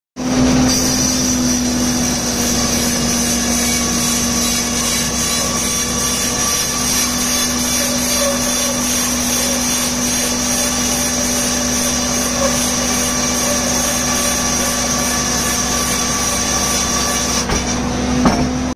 Playing with a big bandsaw mill today. Milling some rock maple. This thing is a monster!
Playing with a huge bandsaw mill.  Milled some rock maple.  This mill is in my friend backyard!  it's the biggest bandsawmill I've used.  Not sure I'll ever use a bigger one then this one.